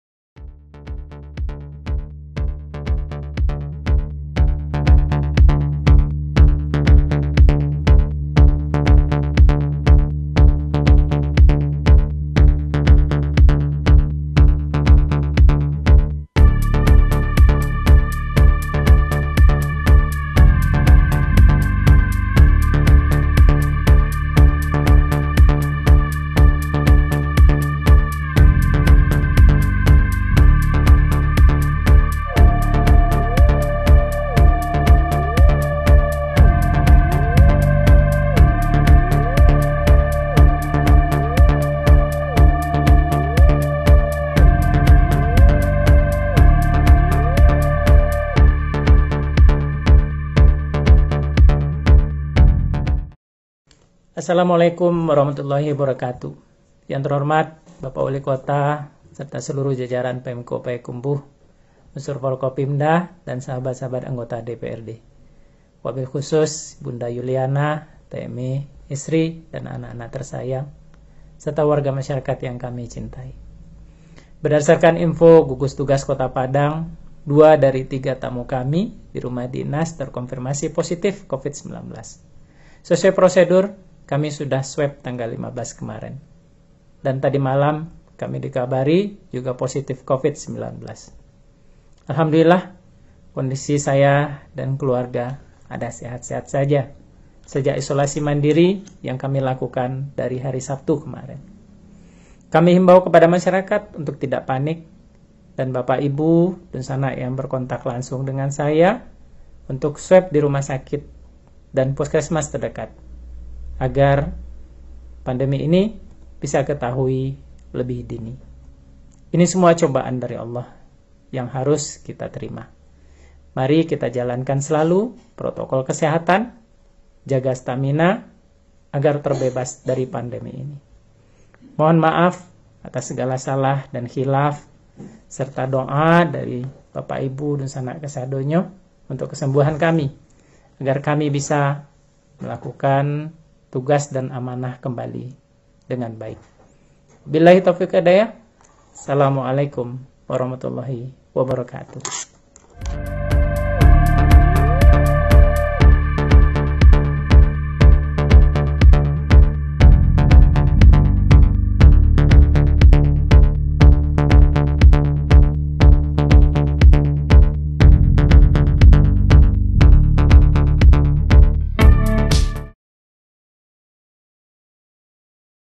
UPDATE!! Wawako Payakumbuh Positif Corona Usai Dikunjungi Teman dari Kota Padang
Wakil Wali Kota (Wawako) Payakumbuh Erwin Yunaz dilaporkan positif terinfeksi Virus Corona (Covid-19) dari hasil pemeriksaan sampel dilaporkan hari ini, Rabu (19/8/2020)